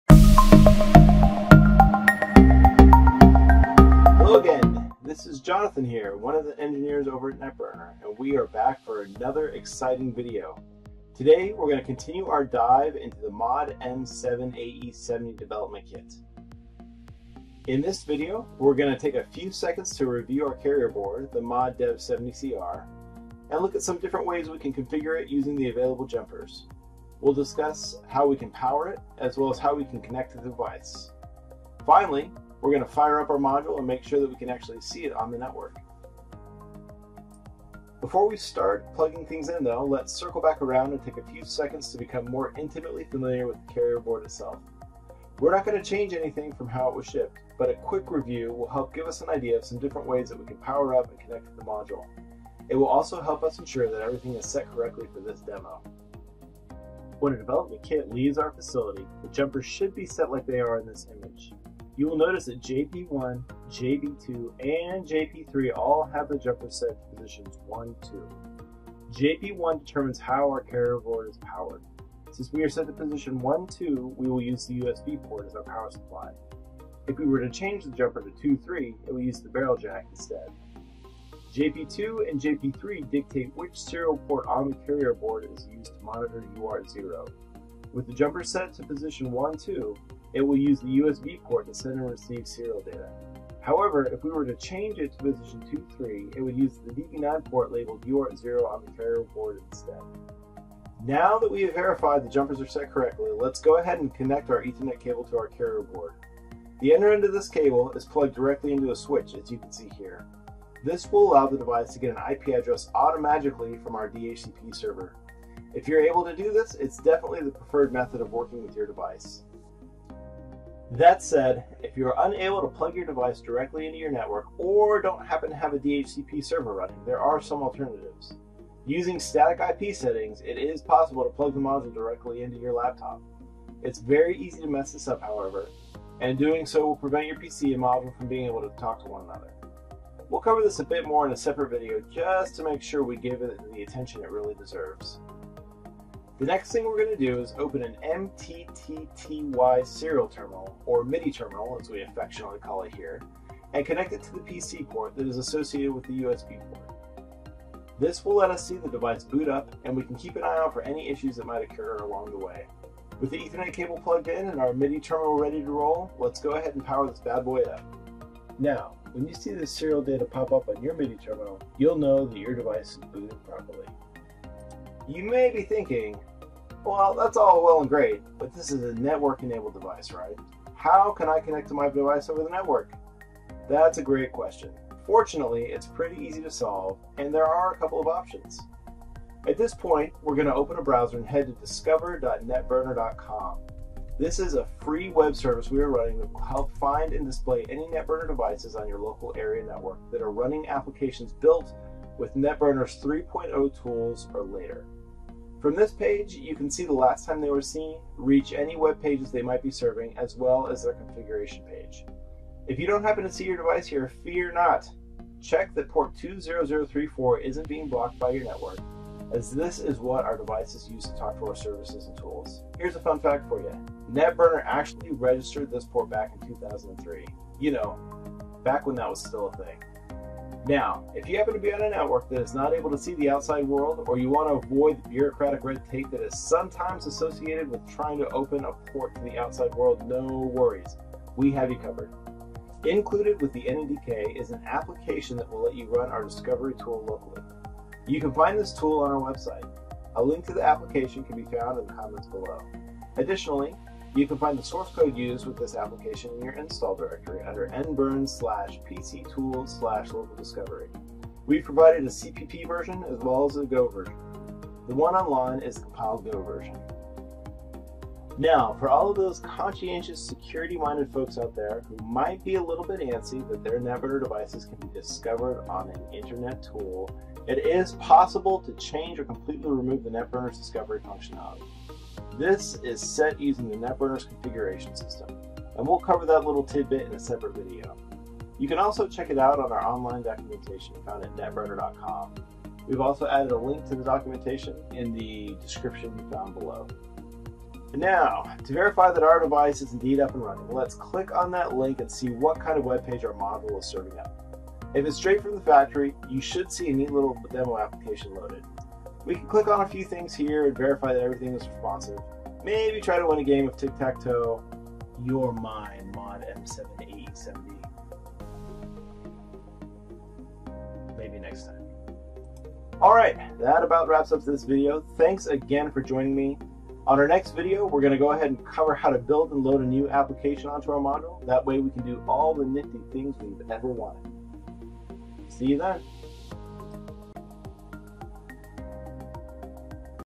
MODM7AE70 Dev Kit Quick Start Guide - Video 2
In this second quick start video, we get rolling with the MODM7AE70 development kit. We start with examining the carrier board, the MOD-DEV-70CR, and continue with powering our device up and finding it on the network.